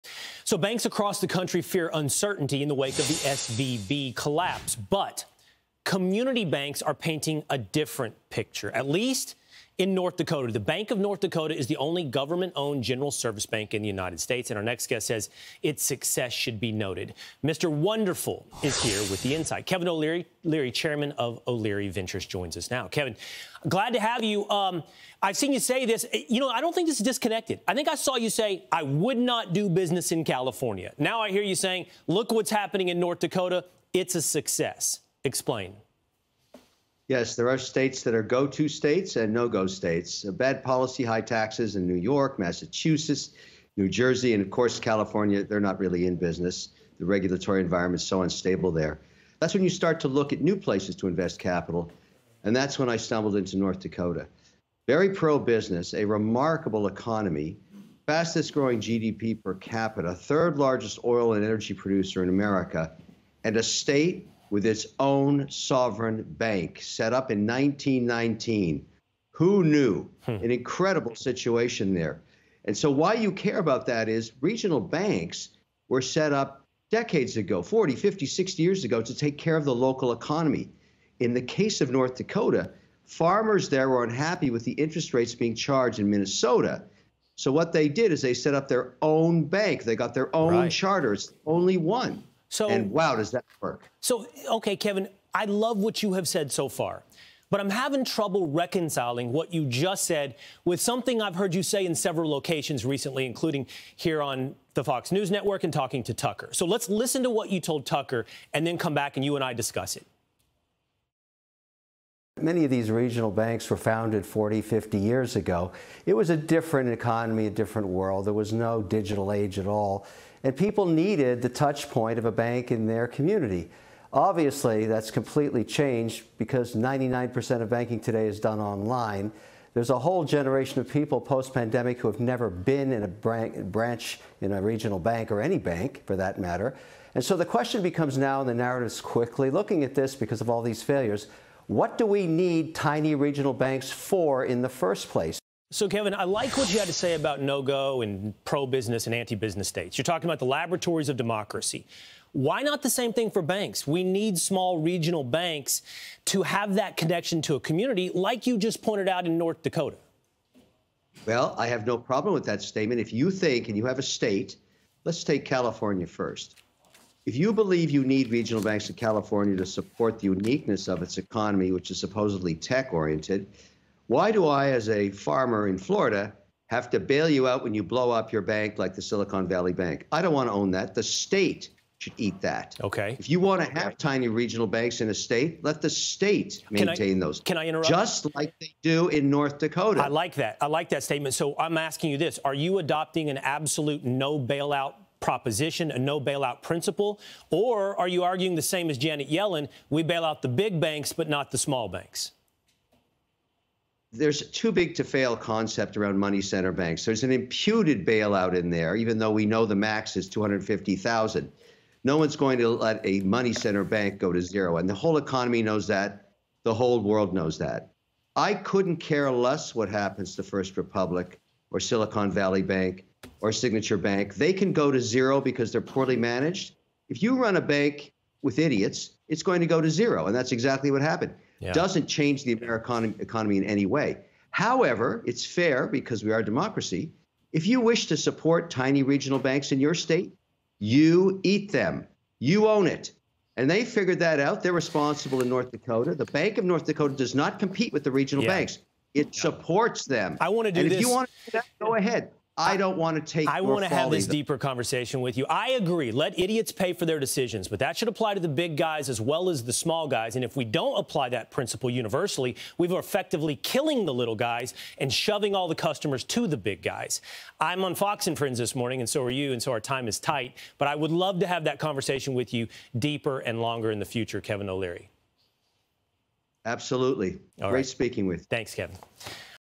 Kevin O'Leary: There's an 'incredible situation' unfolding here
'Shark Tank' star Kevin O'Leary explains the success behind North Dakota's public banks and the impact of the SVB collapse on regional banks.